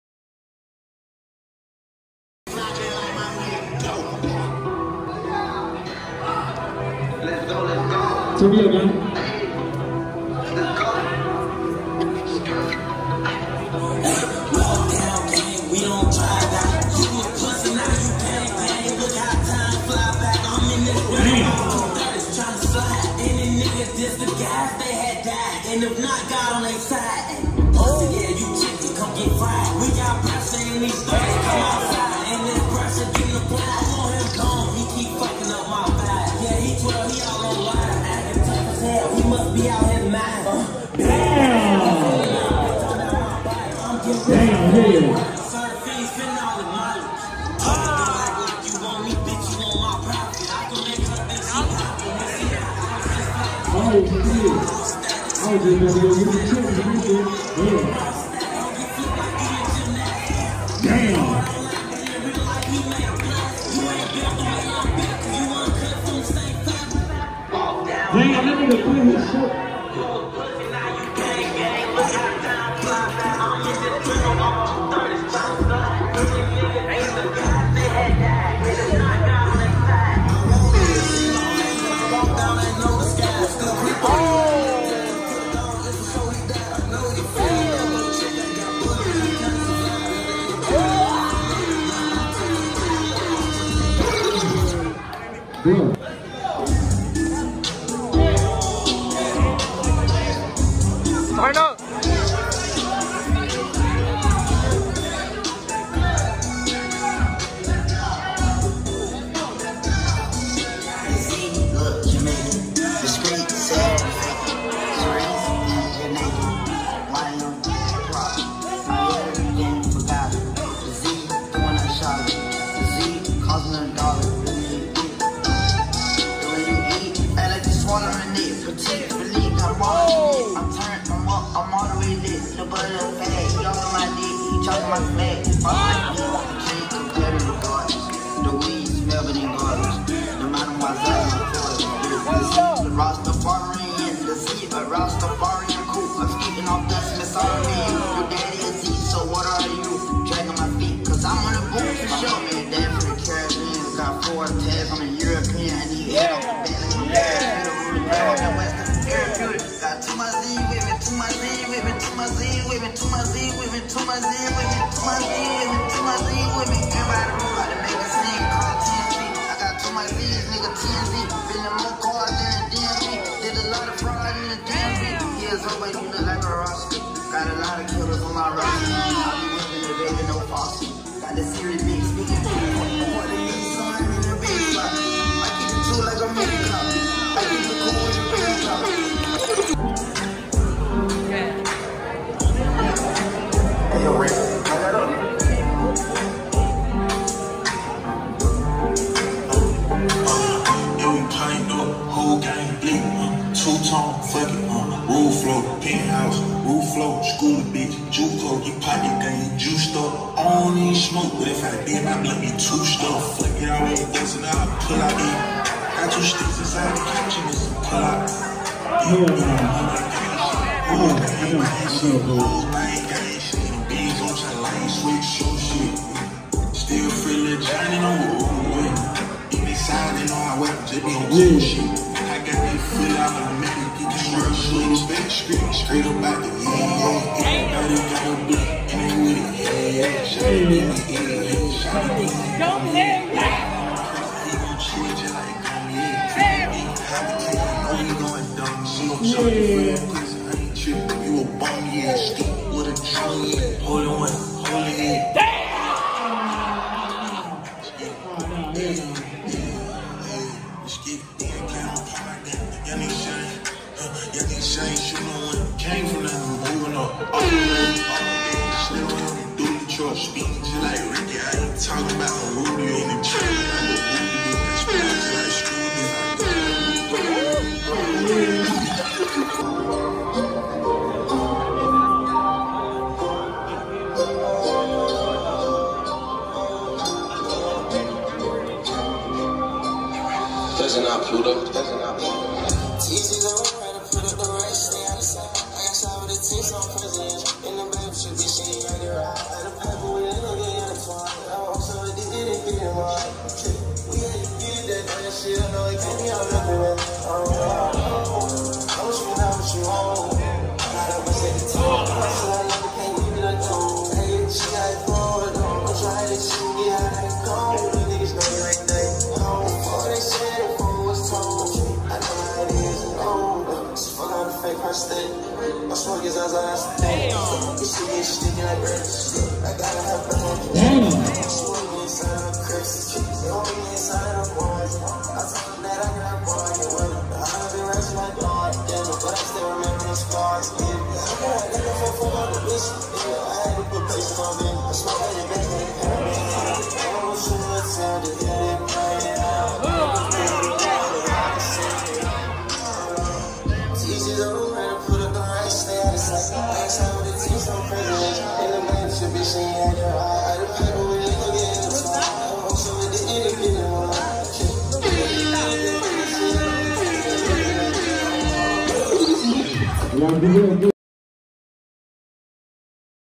BOTG 4 Gorilla Warfare Tournament Pt 1
BATTLE OF THE GORILLAS 4 Gorilla Warfare 🦍🔥
BIGGEST TREND DANCE BATTLE EVENT‼️
POWERED BY: @theylovebigmatt